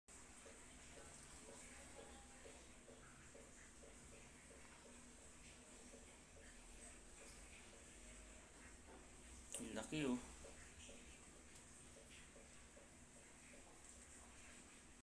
HUNTSMAN SPIDER. SINO ANG MERON NITO SA BAHAY?"Huntsman Spider in My House!
SHORTVIDEO
Join me on a thrilling journey as we explore the unexpected presence of a Huntsman Spider right in my own home! In this video, we'll delve into the intriguing world of these arachnid visitors, discussing their habits, characteristics, and the benefits they bring to our living spaces.

They are also called giant crab spiders because of their size and appearance.